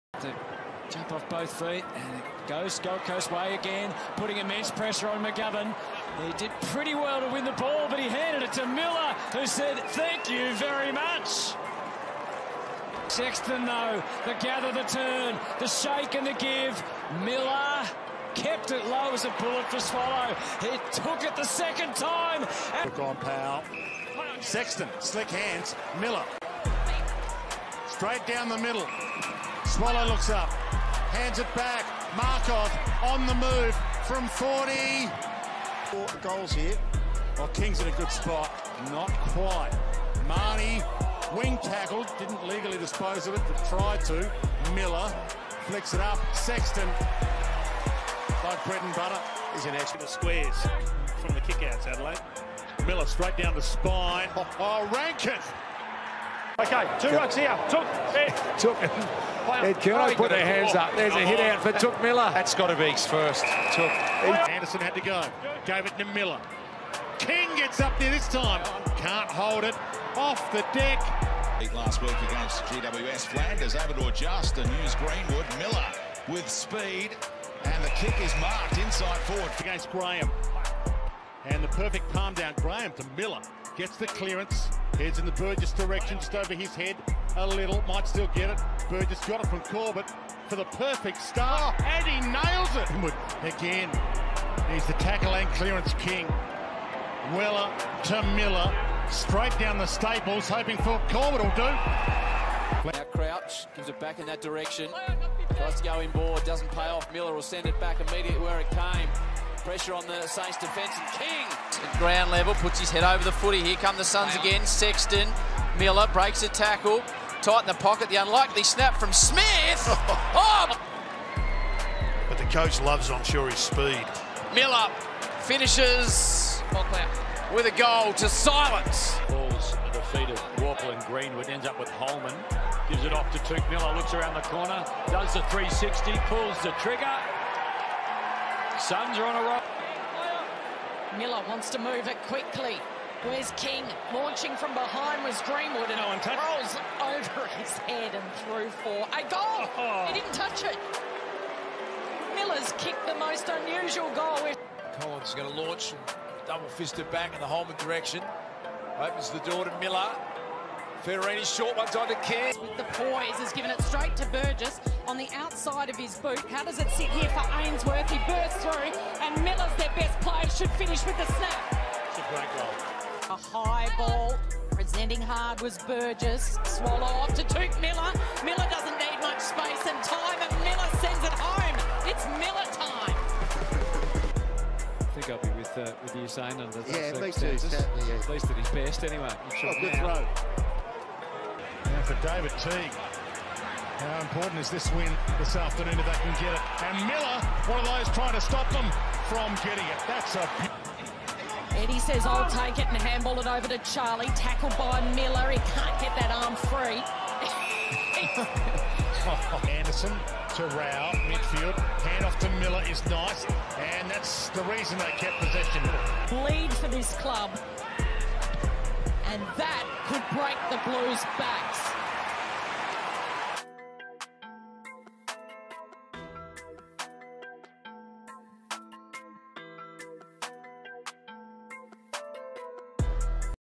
Touk Miller 2021 Highlight Reel
Games: 21
Averages: 31.8 disposals, 4.5 marks, 7.1 tackles, 4.3 inside 50s, 5.7 clearances per game.

Beat by Unknown Instrumentalz (youtube)